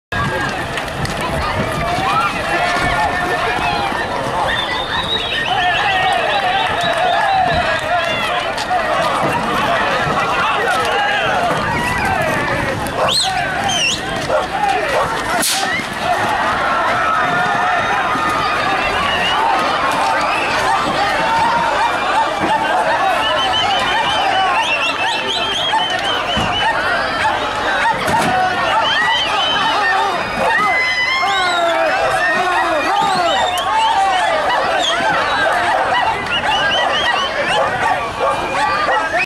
Bull Destroys 4x4 || ViralHog
Occurred on July 1, 2016 / Peñalver, Guadalajara, Spain

A bull runs through the streets of Spain and charges a 4x4 vehicle.